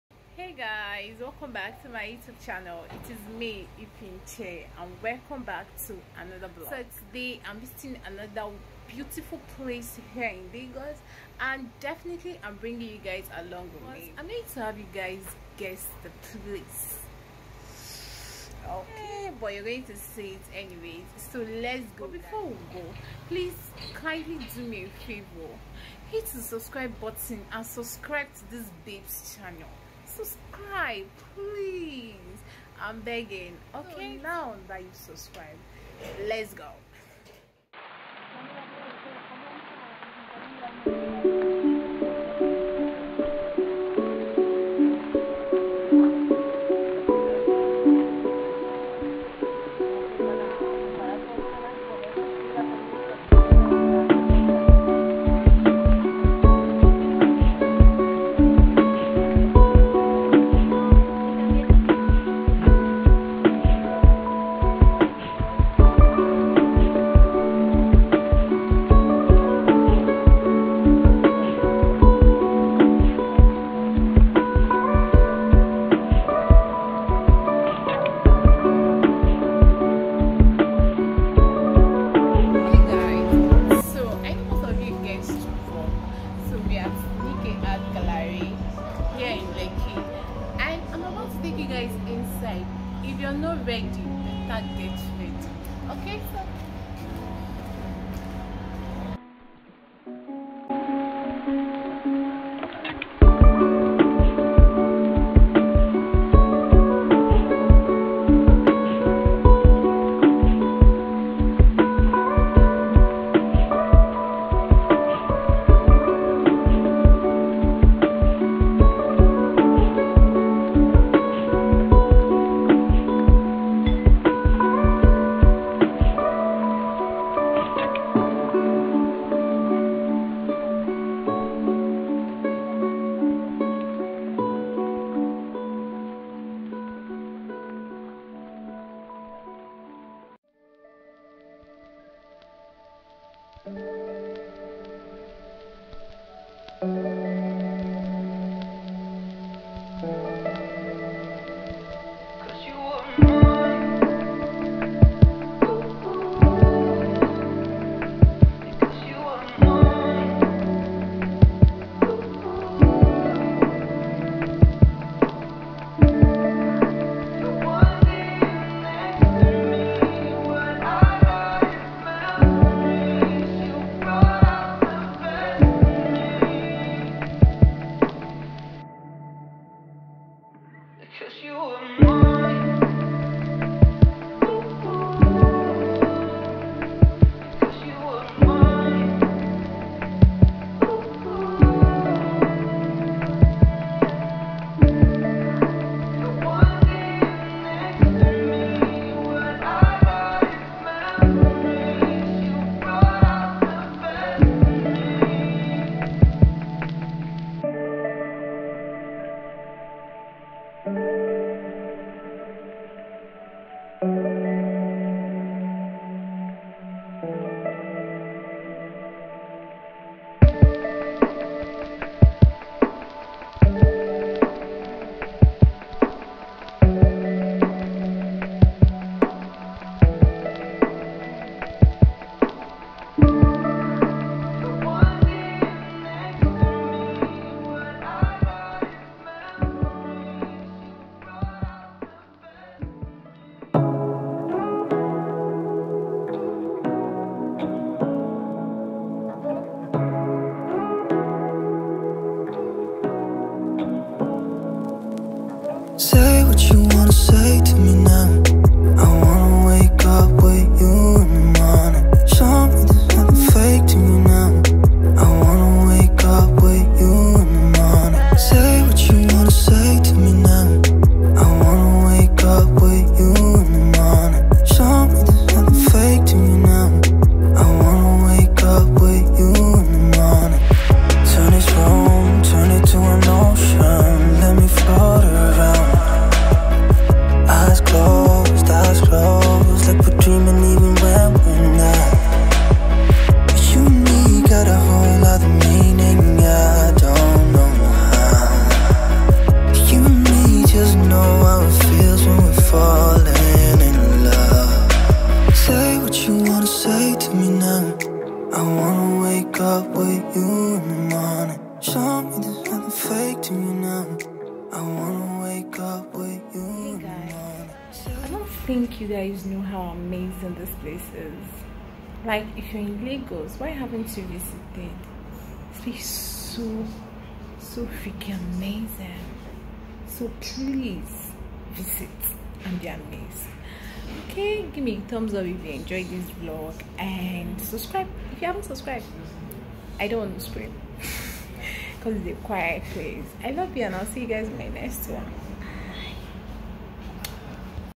Lagosliving #3: Visiting The Biggest Art Gallery In The World | Nike Art Gallery | Gallery Tour
I had fun touring this house art gallery and I hope you guys are amazed by what you see?